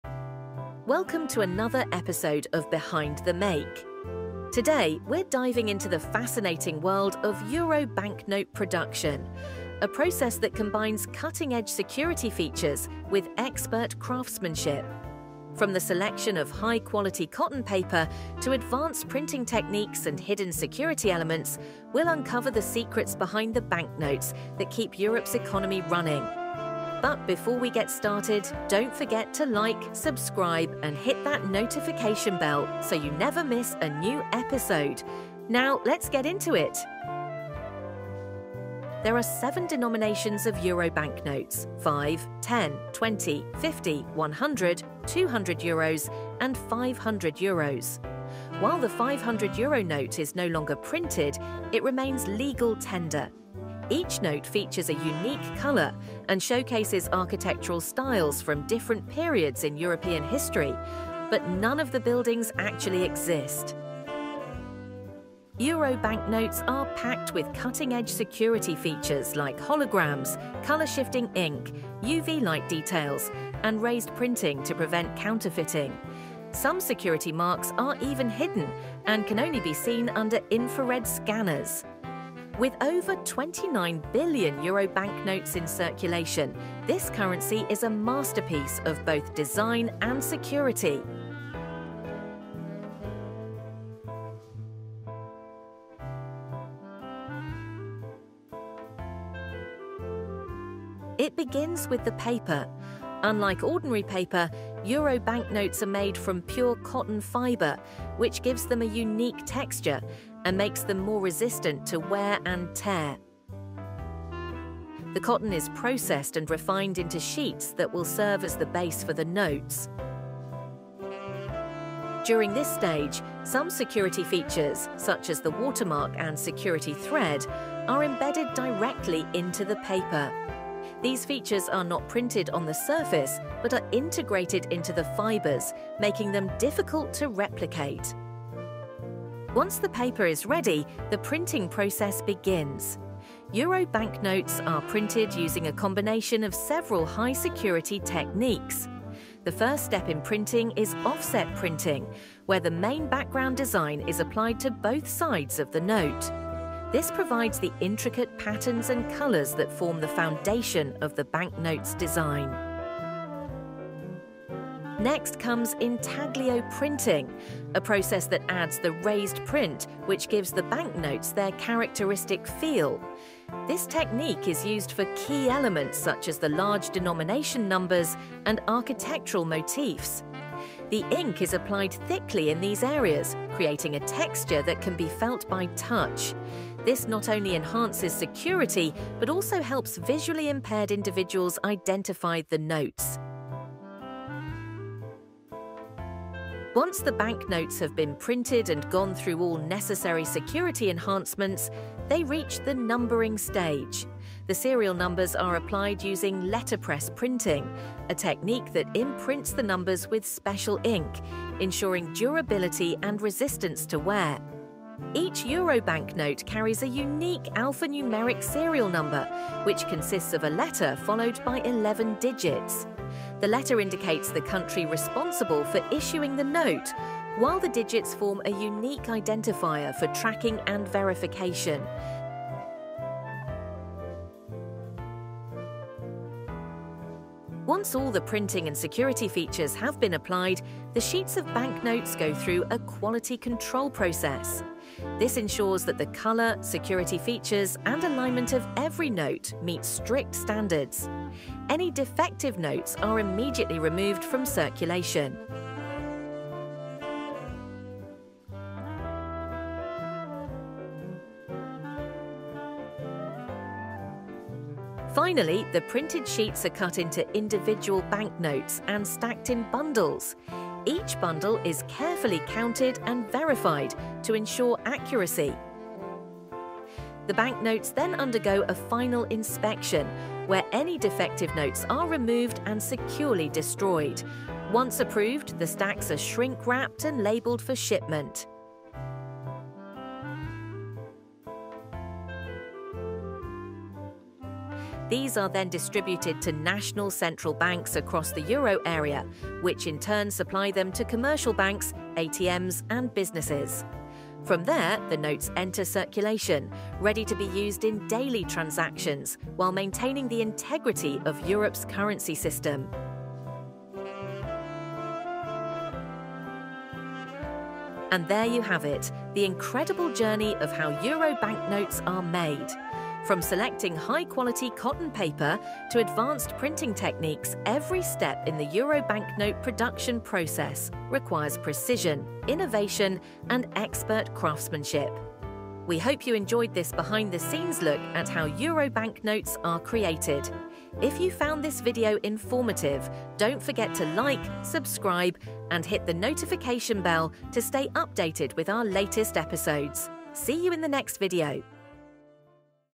Euro Baknotes: How Its Made 💶
Have you ever wondered how Euro banknotes are made? In this episode of Behind The Make, we take you inside the high-tech world of currency production, where craftsmanship meets cutting-edge security.

From high-quality cotton paper to advanced printing techniques, we uncover the incredible journey of how Euro banknotes are created. Learn about the hidden security features, microprinting, holograms, and color-changing inks that make counterfeiting nearly impossible. Plus, find out what those serial numbers really mean!

Now, let’s dive into the fascinating process behind the money in your pocket! 💶💰